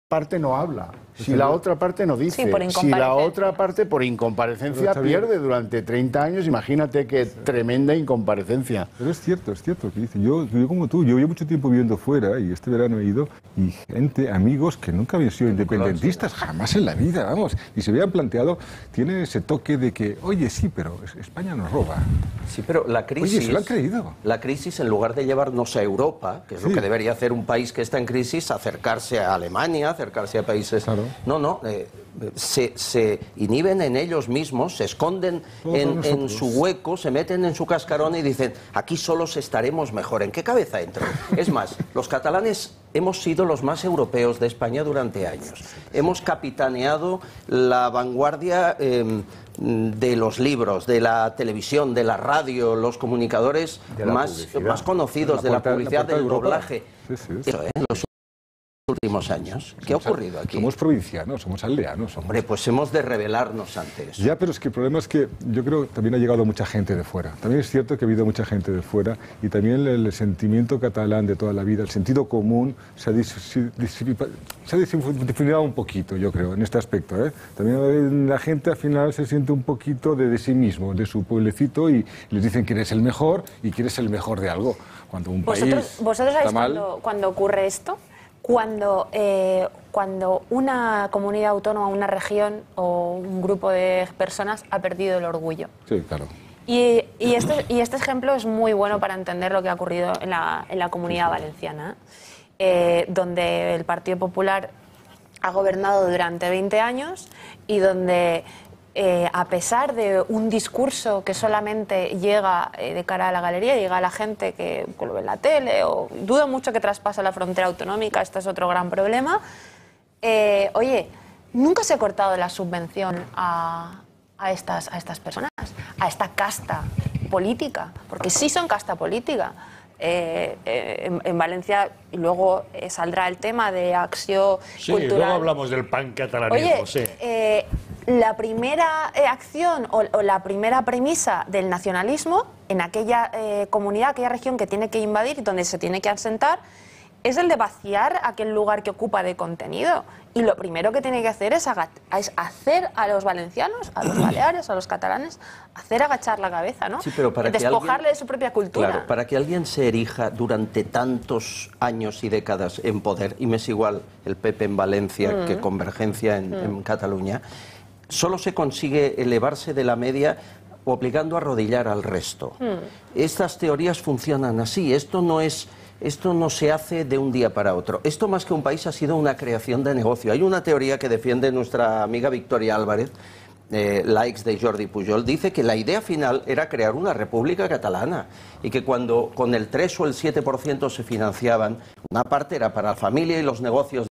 'Tengo amigos que no eran independentistas y que se han creído lo de que España nos roba'
'Los independentistas son provincianos, son aldeanos'.